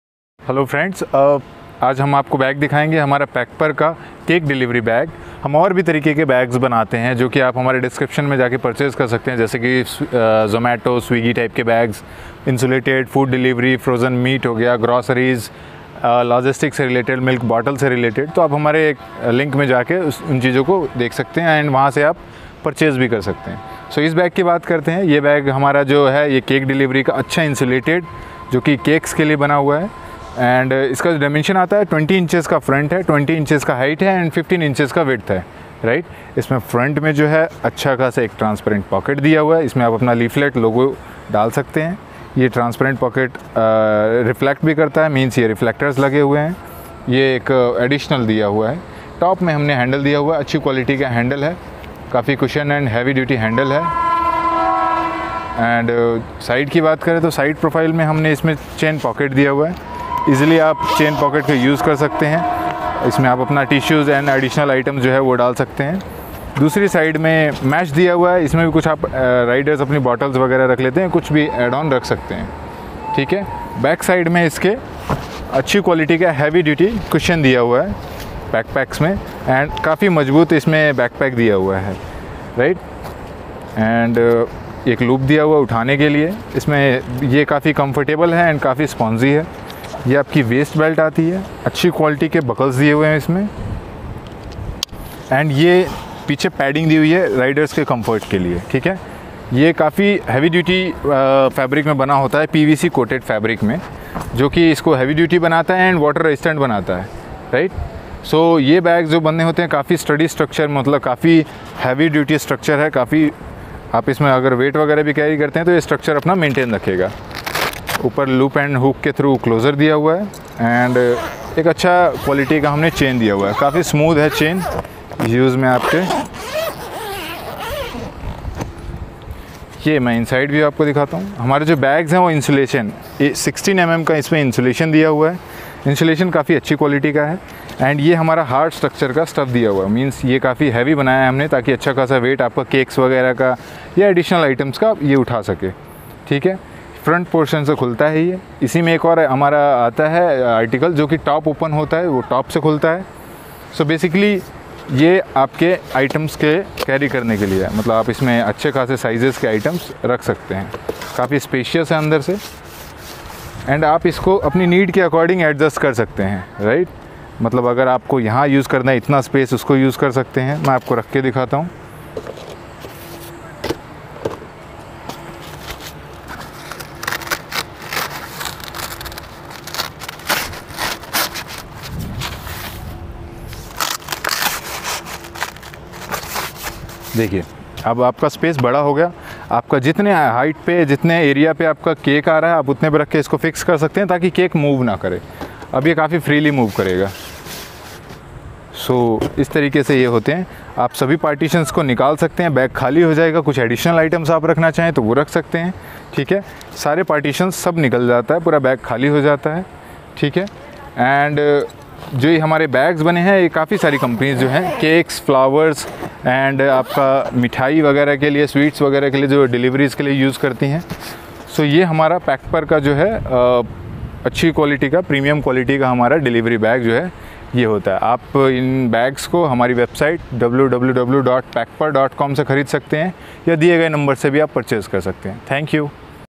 Packpr Cake / food delivery bag front open
This Packpr Cake delivery bag gives you provision to load your cake from front side.
It's hard structured wall makes bag very sturdy to carry sensitive items like gifts, flowers etc.
16 mm epe insulation all around the bag keeps the item at constant temperature to a certain time.